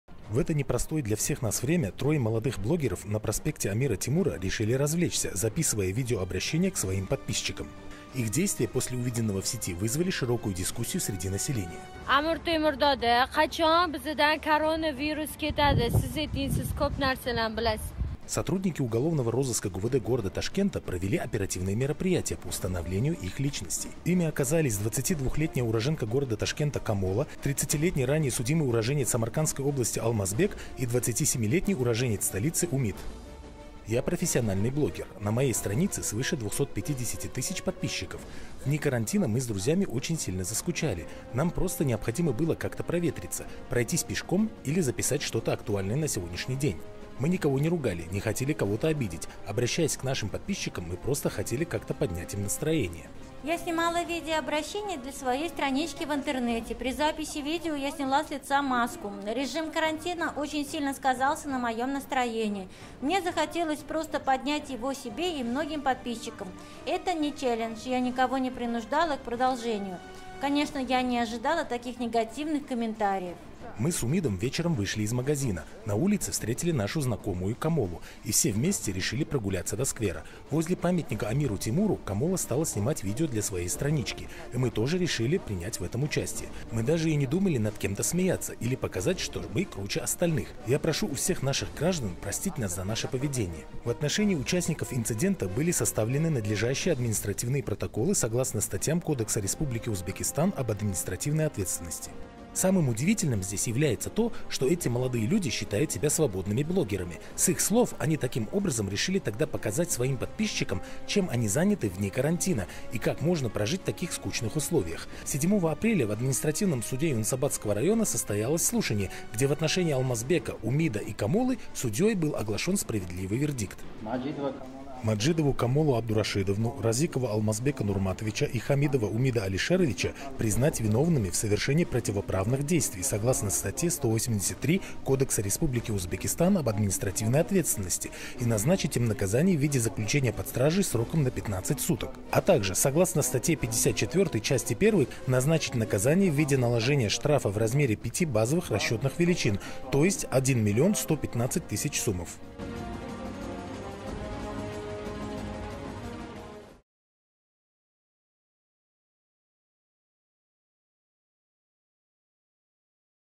Суд арестовал блогеров, снявших видео на сквере Амира Темура в Ташкенте
Трое молодых блогеров были арестованы на 15 суток за нарушение правил карантина и мелкое хулиганство. Они сняли видео у памятника Амиру Темуру, находясь в общественном месте без защитных масок.